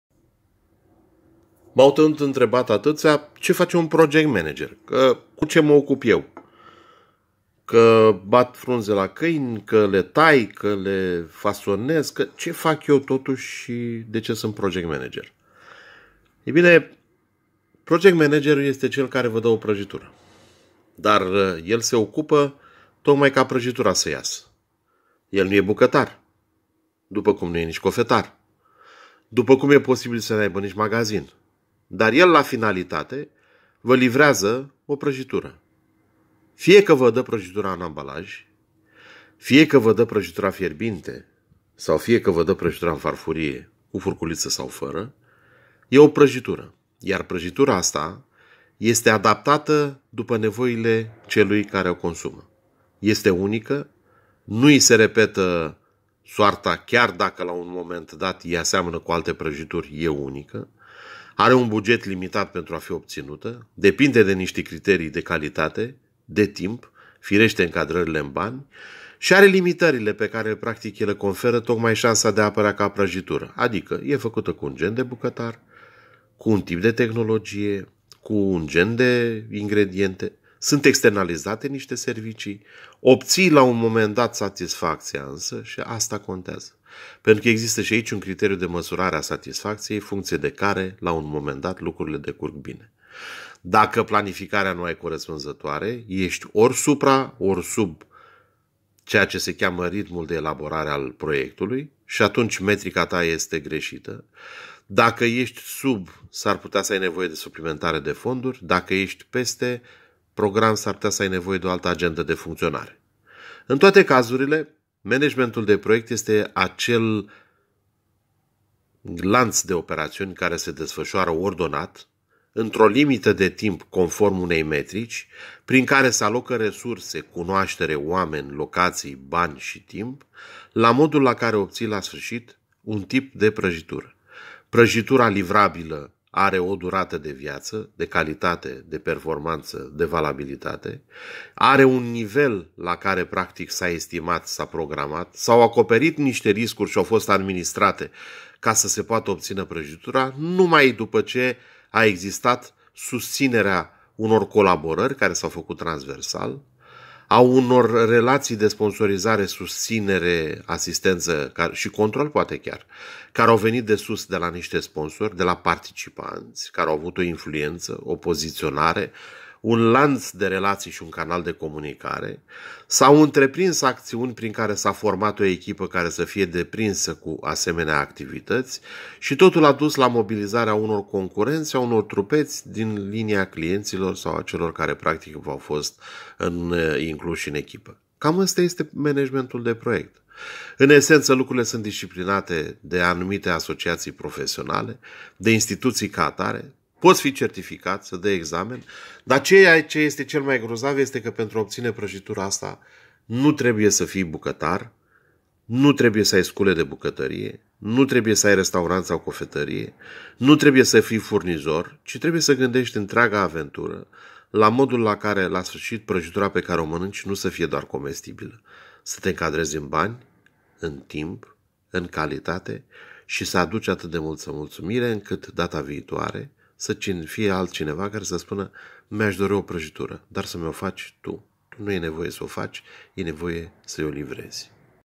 Project Management , o definiție
Project Management, o disciplină fun a carei deprindere de noțiuni si tehnici poți face si obține aproape orice.